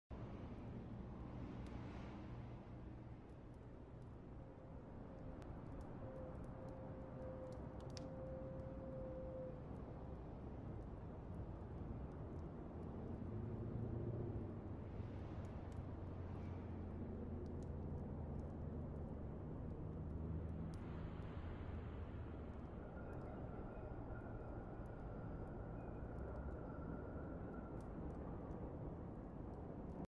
Dark Alley - Unity5 (showreel)
Scene of dark back alley made in Unity 5.4.2f2 and 3dsMax